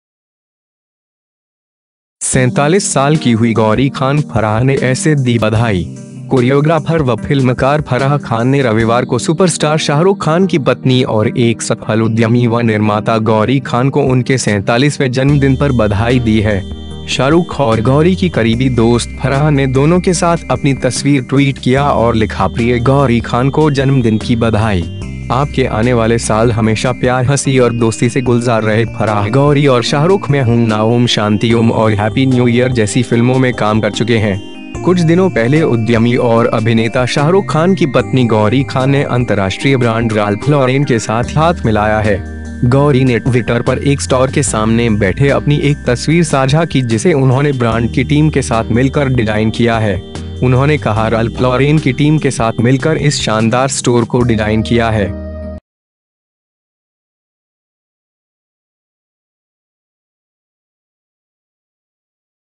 47 साल की हुई गौरी खान, फराह ने ऐसे दी बधाई
47 साल की हुई गौरी खान, फराह ने ऐसे दी बधाई
 कोरियोग्राफर व फिल्मकार फराह खान ने रविवार को सुपरस्टार शाहरुख खान की पत्नी और एक सफल उद्यमी व निर्माता गौरी खान को उनके 47वें जन्मदिन पर बधाई दी है।

शाहरुख और गौरी की करीबी दोस्त फराह ने दोनों के साथ अपनी तस्वीर ट्वीट किया और लिखा, ‘‘प्रिय गौरी खान को जन्मदिन की बधाई। आपके आने वाले साल हमेशा प्यार, हंसी और दोस्ती से गुलजार रहे।’’ फराह, गौरी और शाहरुख ‘मैं हूं ना’, ‘ओम शांति ओम’ और ‘हैप्पी न्यू ईयर’ जैसी फिल्मों में काम कर चुके हैं।  कुछ दिनों पहले उद्यमी और अभिनेता शाहरुख खान की पत्नी गौरी खान ने अंतर्राष्ट्रीय ब्रांड राल्फ लॉरेन के साथ हाथ मिलाया है। गौरी ने ट्विटर पर एक स्टोर के सामने बैठे अपनी एक तस्वीर साझा की, जिसे उन्होंने ब्रांड की टीम के साथ मिलकर डिजाइन किया है। उन्होंने कहा, राल्फ लॉरेन की टीम के साथ मिलकर इस शानदार स्टोर को डिजाइन किया है।